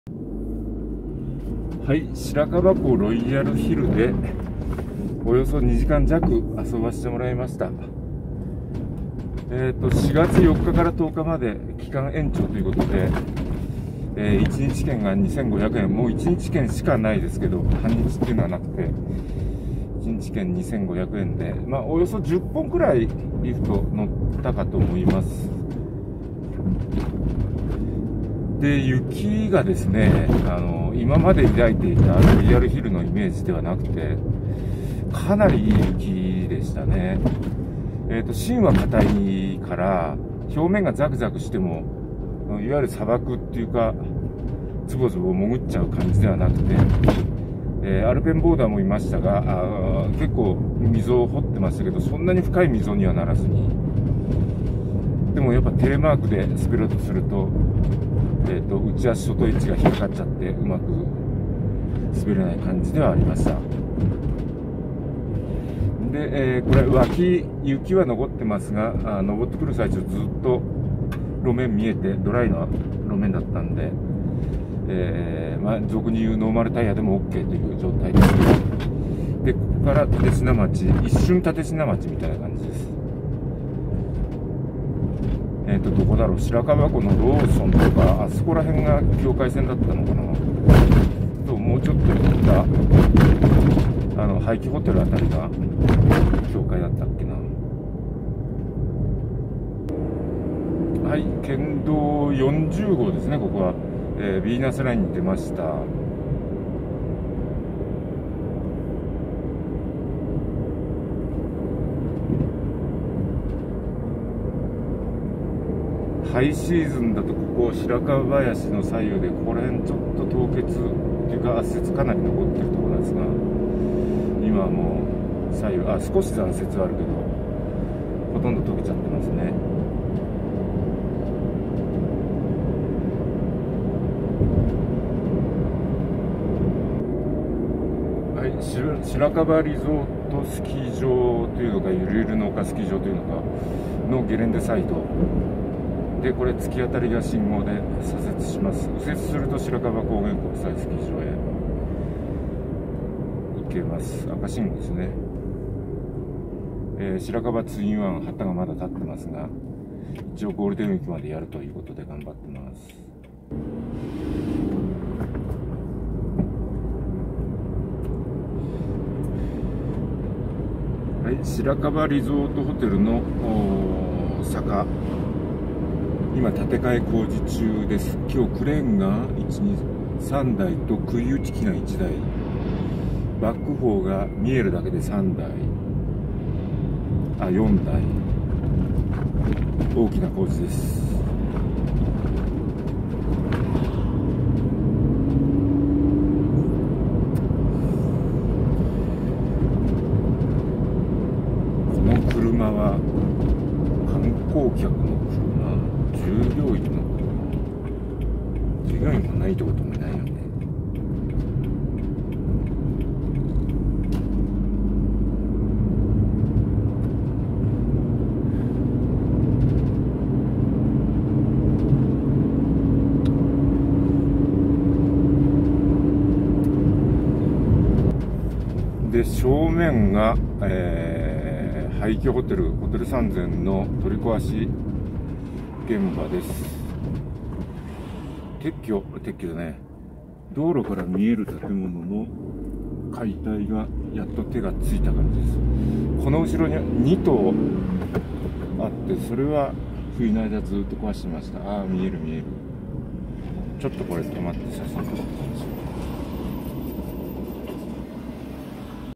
20220408白樺湖ロイヤルヒル1117～帰路路面状況・県道40号
ロイヤルヒルの上の駐車場から白樺湖R152交差点まで。